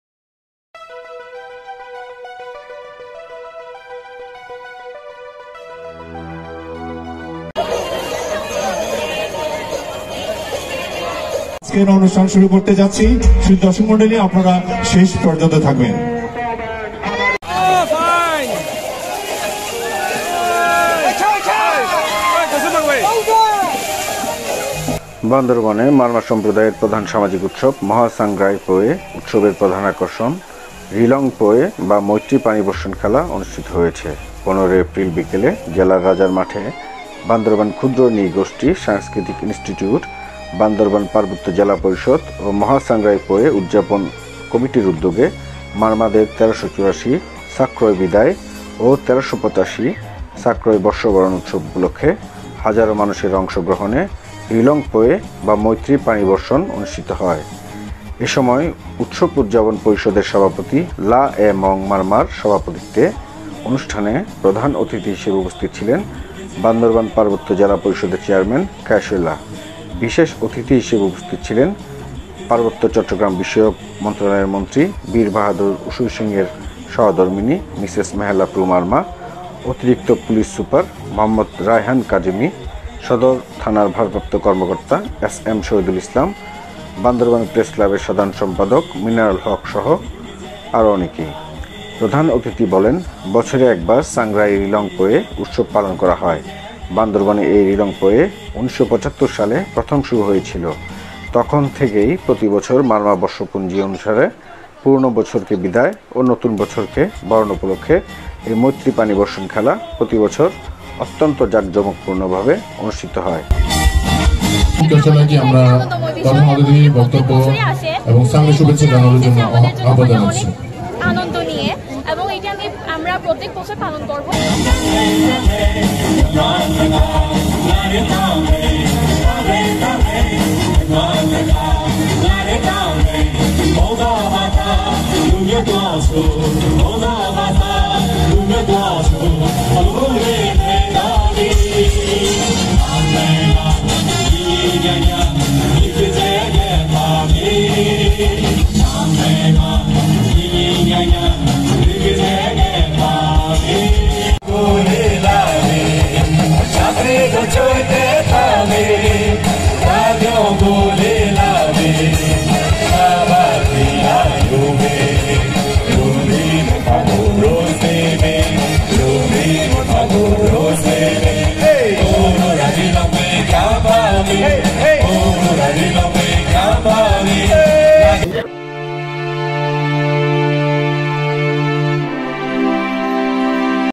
মহা সাংগ্রাইং পোয়েঃ।।SANGARI।।2023।।BANDARBAN
১৫ই এপ্রিল (শনিবার) বিকেলে জেলার রাজার মাঠে বান্দরবান ক্ষুদ্র নৃ-গোষ্ঠীর সাংস্কৃতিক ইনস্টিটিউট, বান্দরবান পার্বত্য জেলা পরিষদ ও মাহাঃ সাংগ্রাইং পোয়েঃ উৎসব উদযাপন কমিটির উদ্যােগে মারমাদের ১৩৮৪ সাক্রই বিদায় ও ১৩৮৫ সাক্রই বর্ষবরন উৎসব উপলক্ষে হাজারো মানুষের অংশ গ্রহণে রিলং পোয়ে (মৈত্রী পানি বর্ষণ খেলা ) অনুষ্ঠান অনুষ্টিত ।